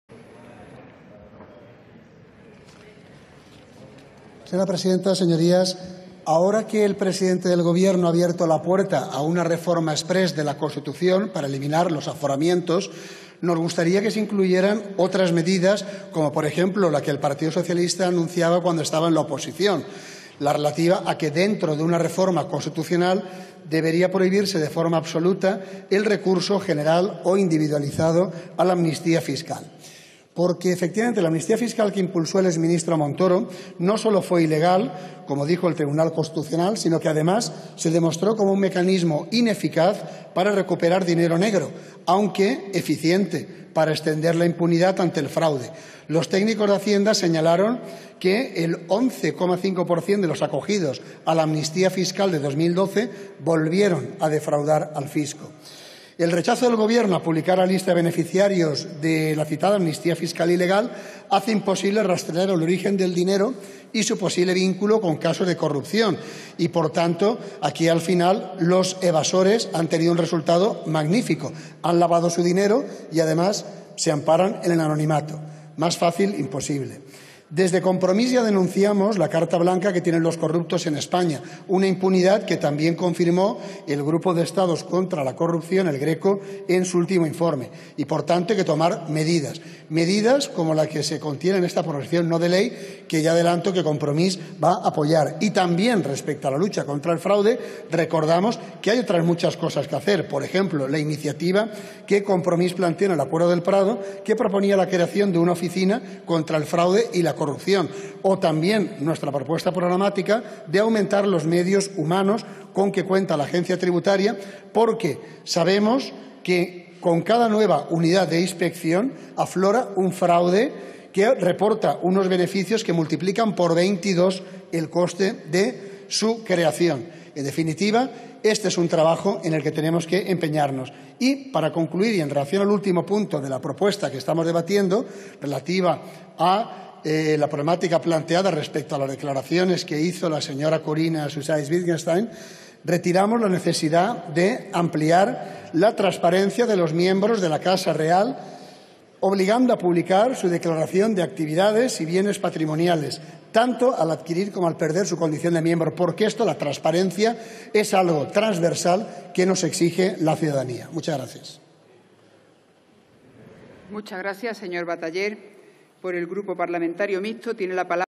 Bataller: "Cal reformar la Constitució per prohibir l'amnistia fiscal"
Intervenció del diputat de Compromís al Congrés, Enric Bataller.